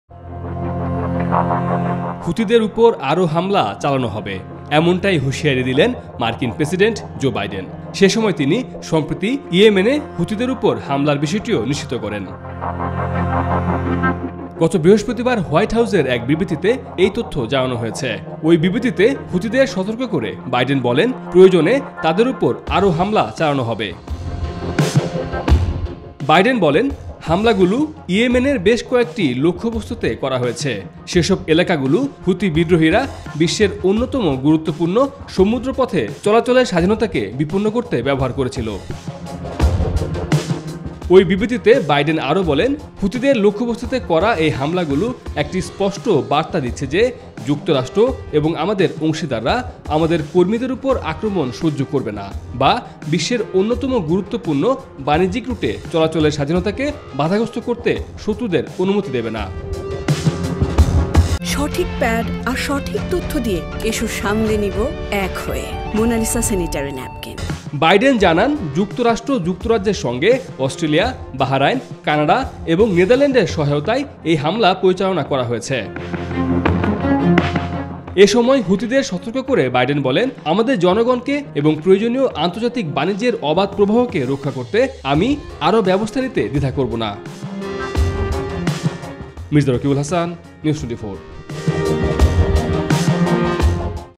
হু* থিদের বাইডেনের সতর্কবার্তা | Red Sea | Joe Biden | United States | News24
হু* থিদের ওপর আরও হা* মলা চালানো হবে। এমনটাই হুশিয়ারি দিলেন মার্কিন প্রেসিডেন্ট জো বাইডেন।  সে সময় তিনি সম্প্রতি ইয়েমেনে হু* থিদের ওপর হা* মলার বিষয়টিও নিশ্চিত করেন।

NEWS24 Team